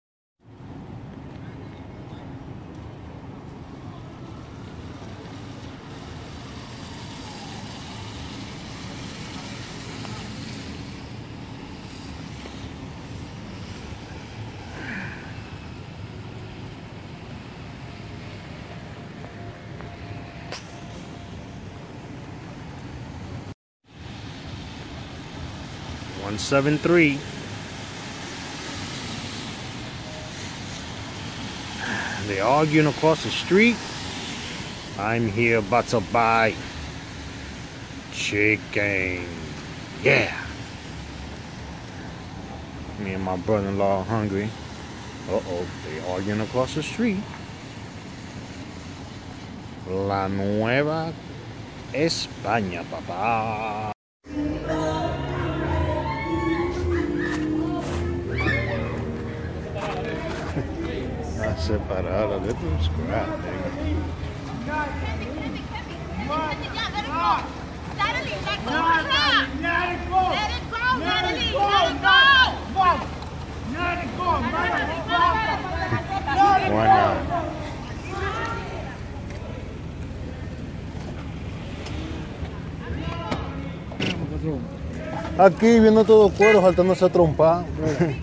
A fight in the fog... Washington Heights nyc
A quick fight... that's all.